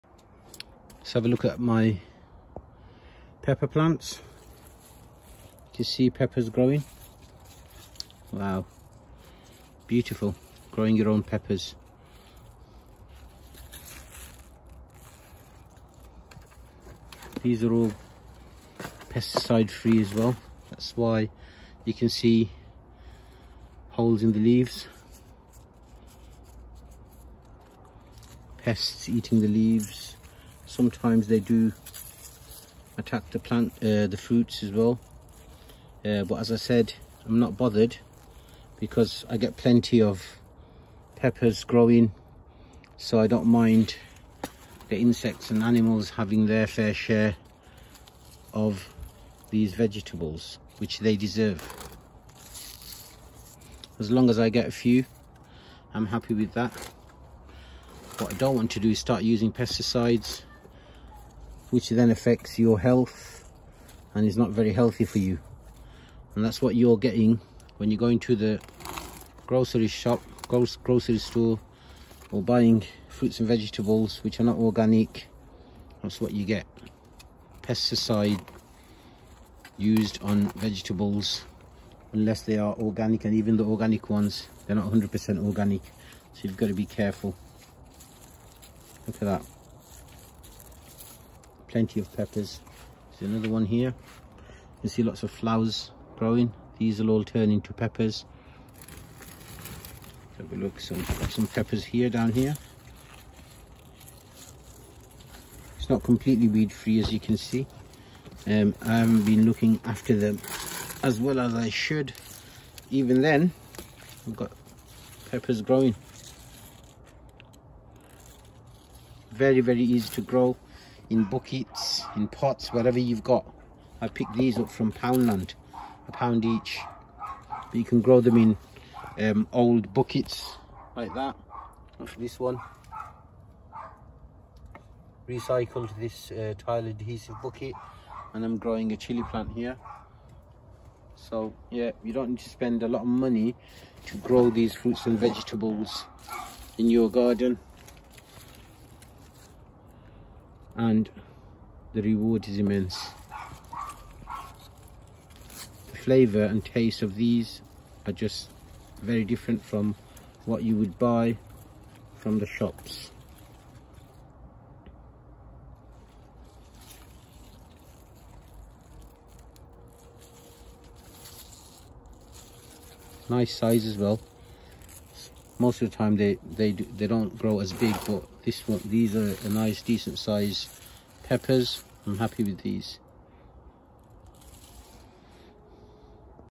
Grow Bell Peppers in Buckets
A short video on growing Bell Peppers in pots or buckets in the garden.